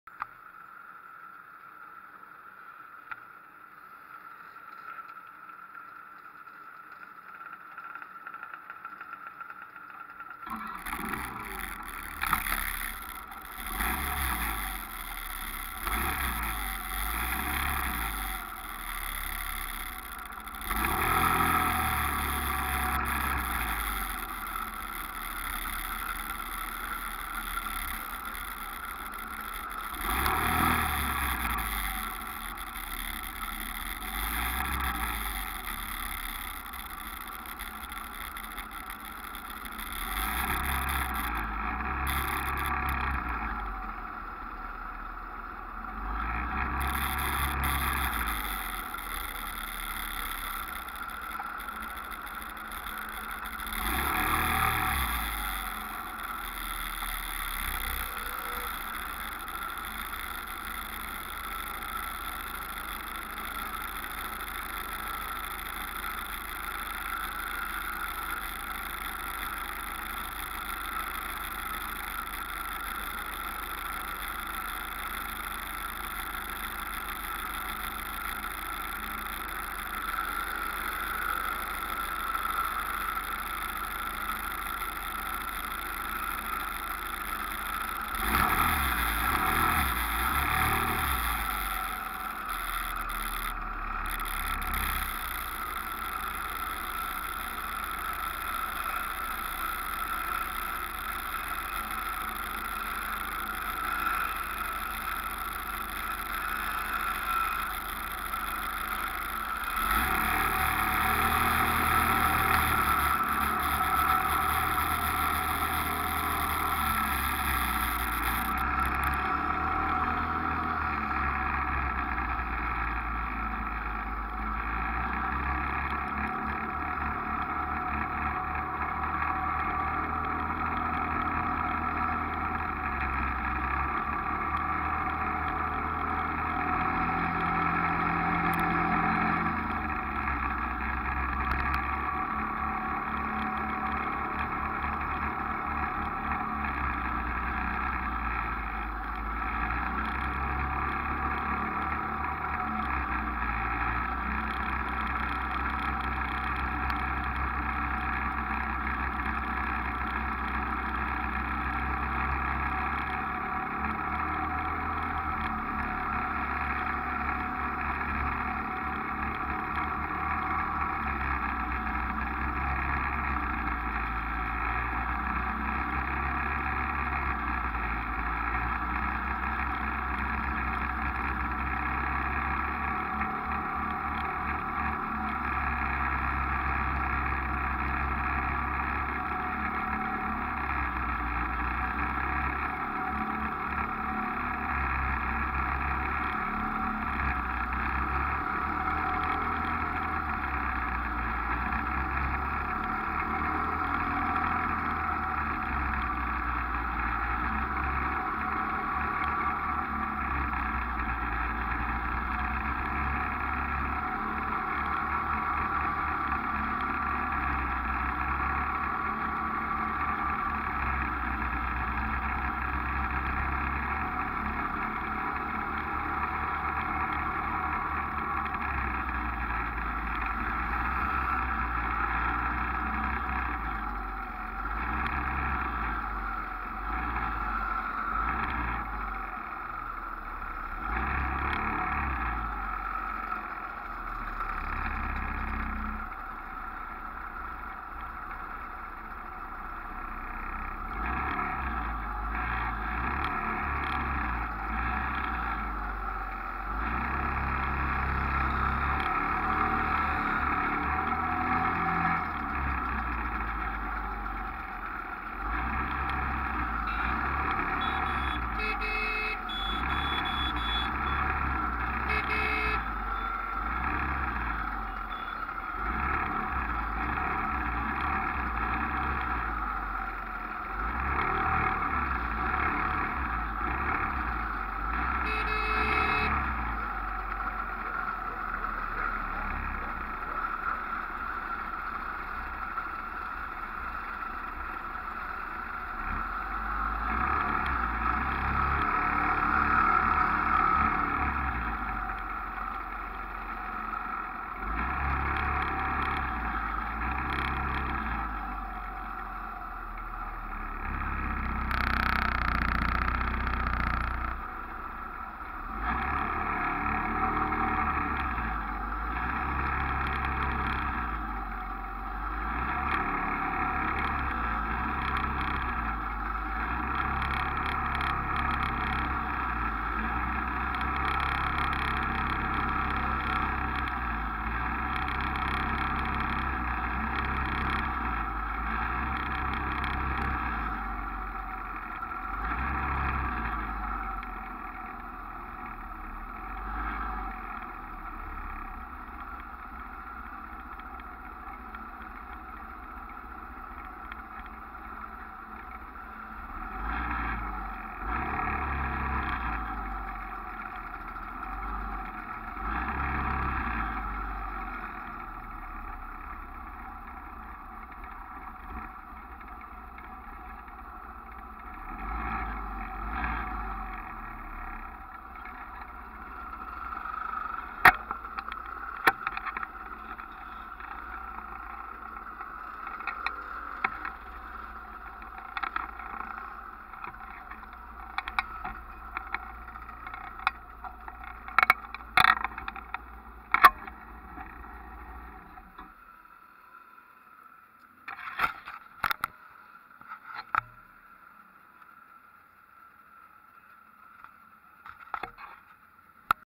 Lee Rigby Ride of Remembrance, 22/05/16, Woolwich Ferry Port to Lee Rigby Memorial.
Great ride out today. Little video from my Vivitar DVR 786hd cam, bit wobbly as was mounted on the rear rack of my Scomadi scooter.
This was shoot from the ferry dock to the site of the Lee Rigby memorial.